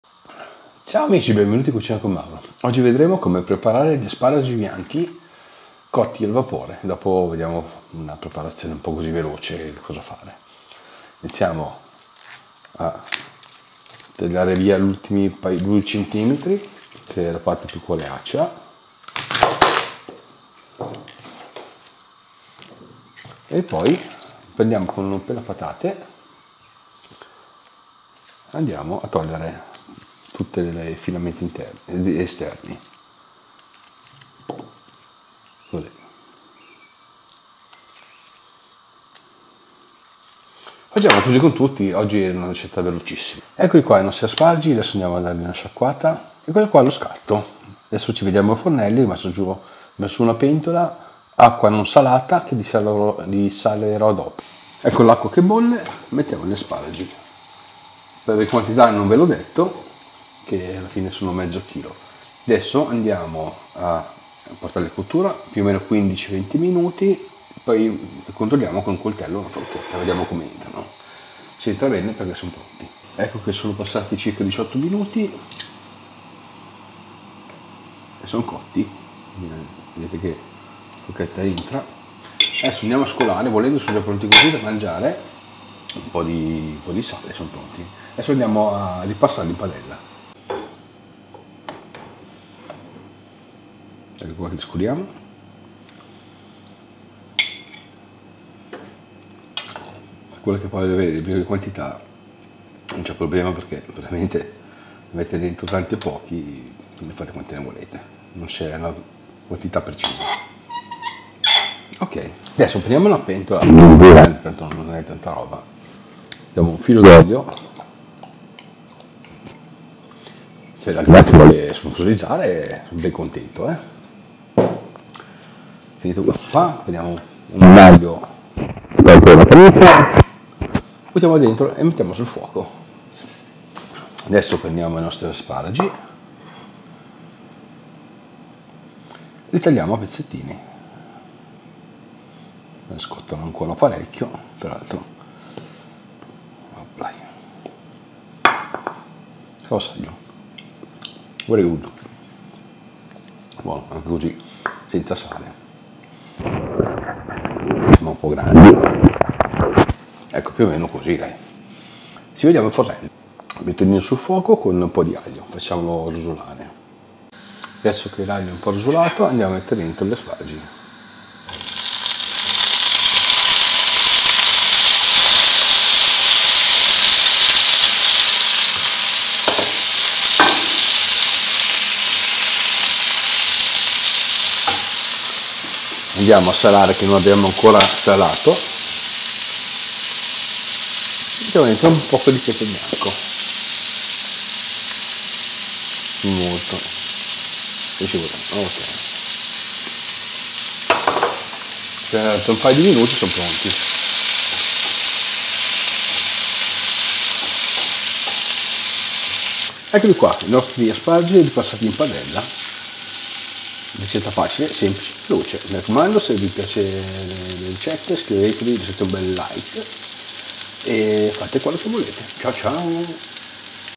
TUTORIAL COME CUOCERE GLI ASPARAGI BIANCHI
INGREDIENTI
asparagi bianchi
aglio
olio sale pepe